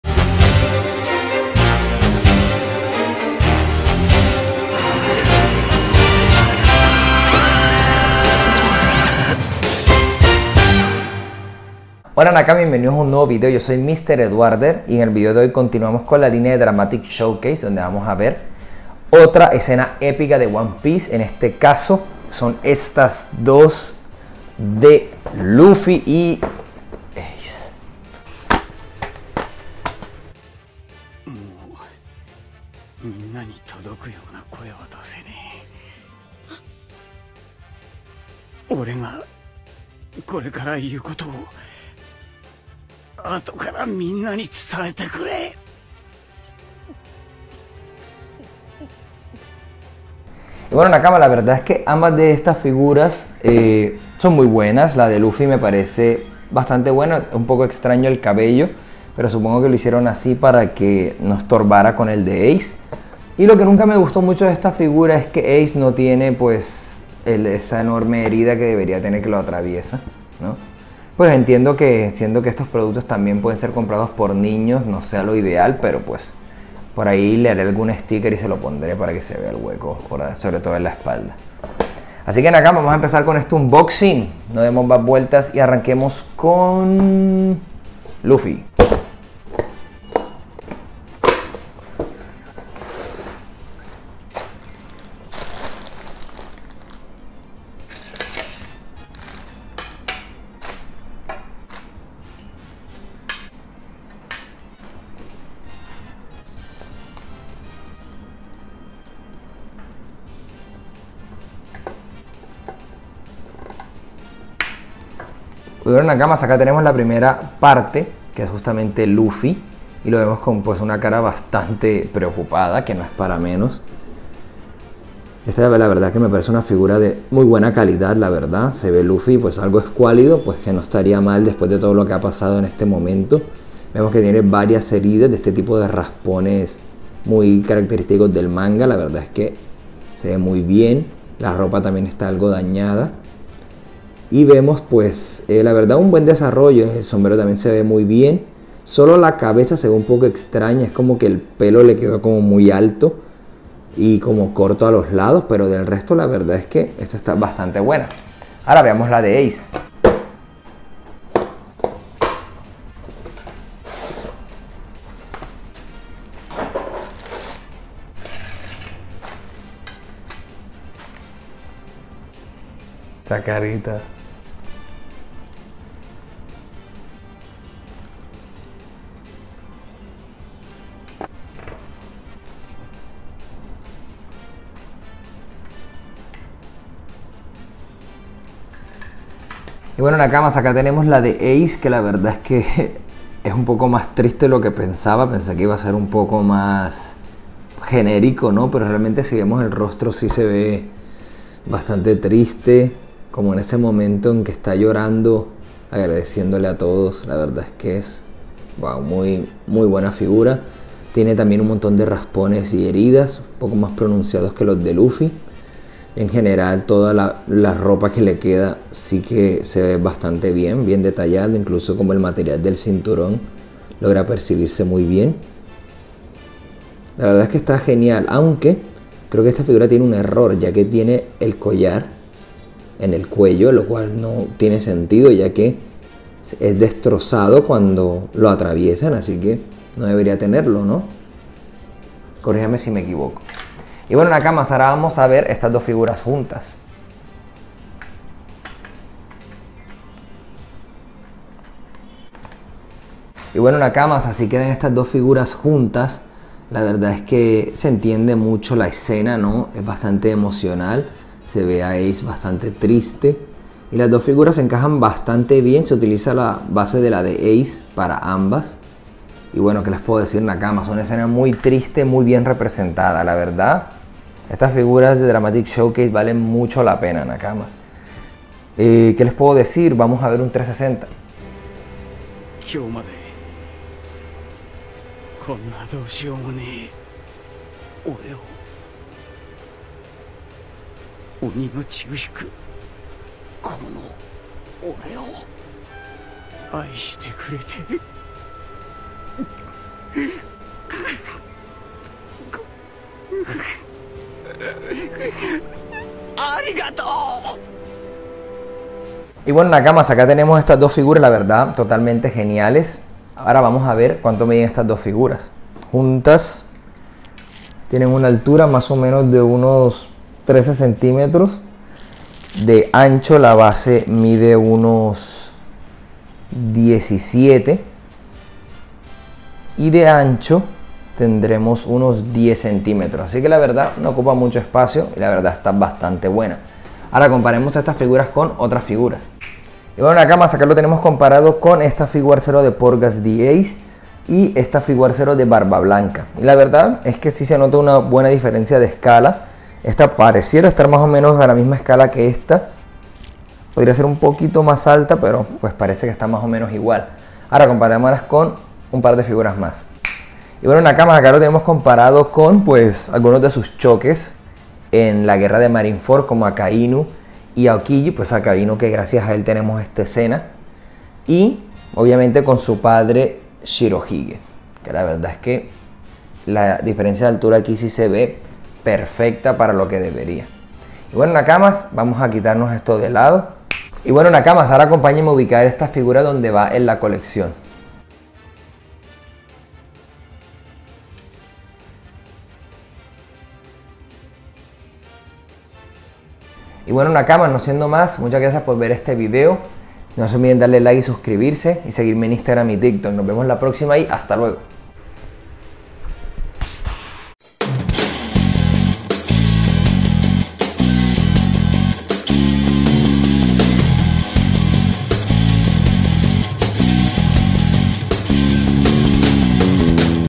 Luffy y Ace - La muerte que cambió todo - Dramatic Showcase 2015 - Unboxing - Banpresto
Vídeo nuevo de nakamas! La muerte de ACE!